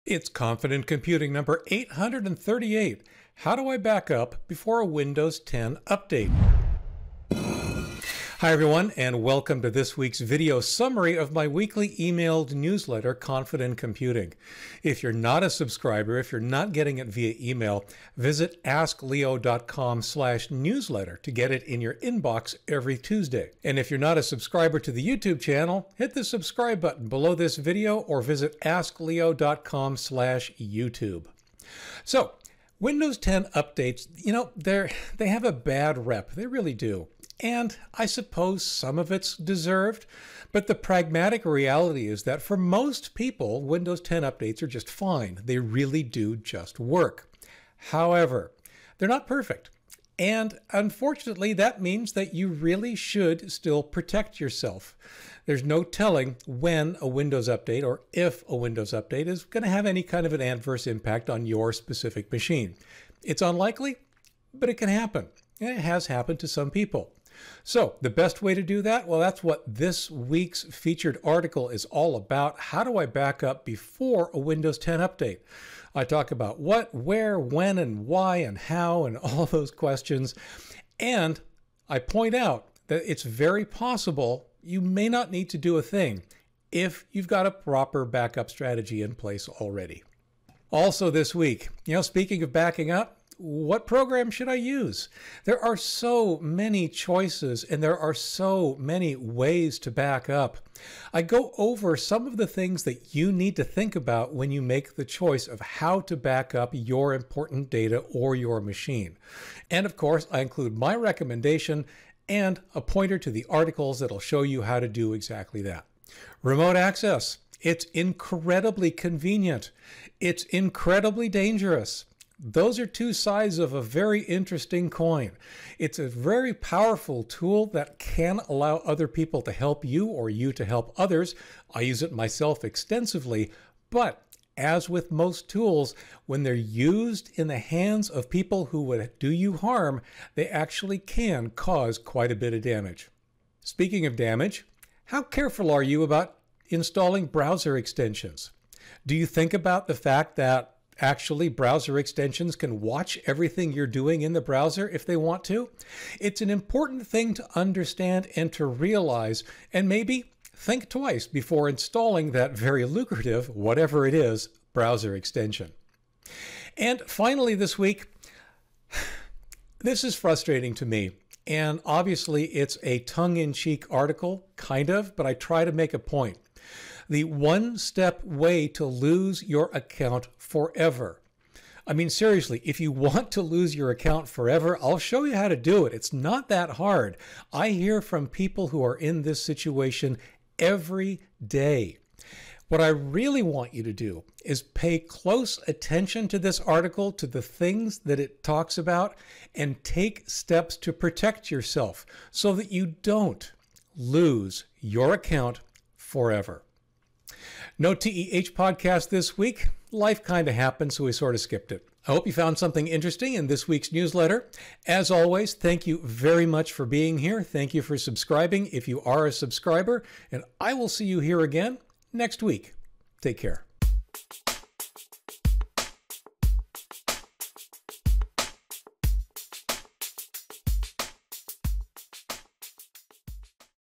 Confident Computing 👍 #838 - How to Back Up Before a Windows 10 Upgrade
Your video summary of this week's Ask Leo! newsletter: Confident Computing #838

This Week's Articles:
✔ What Backup Program Should I Use?
✔ Is it Safe to Allow Remote Access?
✔ Can I Trust Browser Extensions?